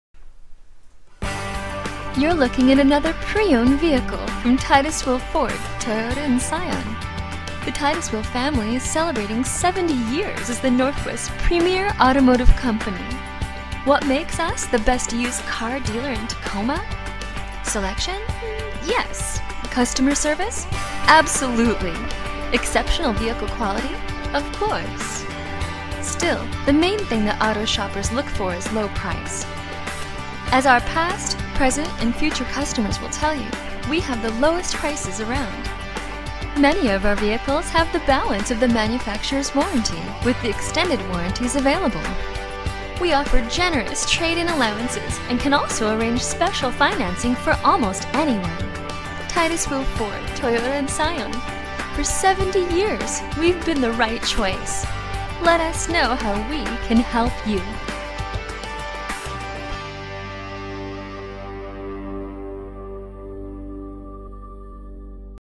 2007 Ford Ranger 2WD, $Call for price at Titus Will Ford in Tacoma, WA
Pick me! Ask for ford used sales when you call.  we have been in business since 1938.  great financing rates available! Center armrest, bed liner, cd player, am/fm radio, split bench seat, cloth seats, warranty, a/c. Call or stop by today. $Call for price
 Titus Will Ford
 3606 S Sprague Avenue